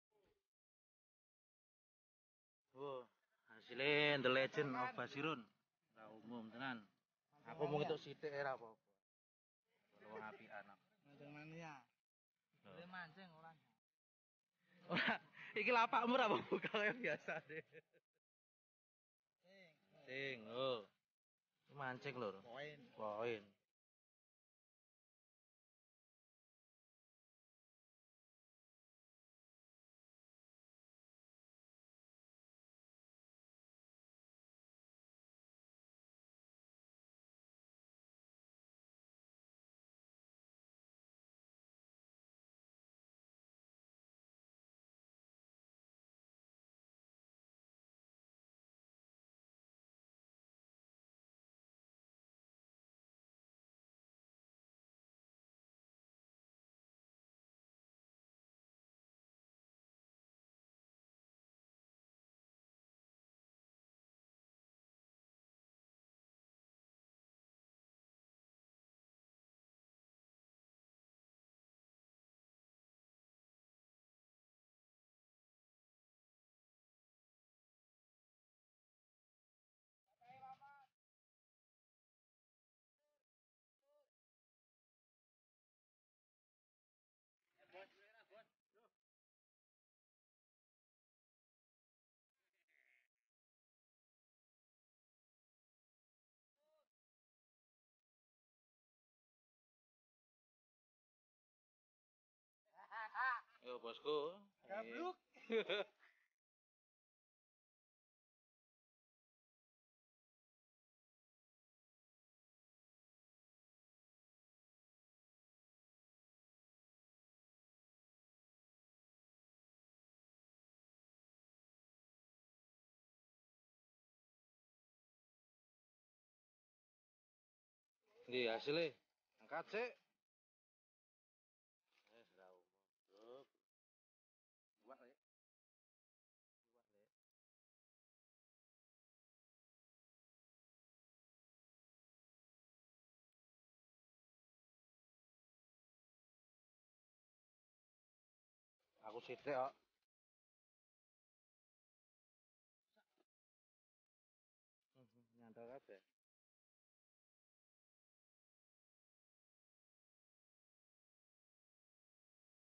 Kuras kolam habis mancing borongan
assalamuallaikum , salam satu hoby, setelah mancing bareng waktunya kuras kolam serbu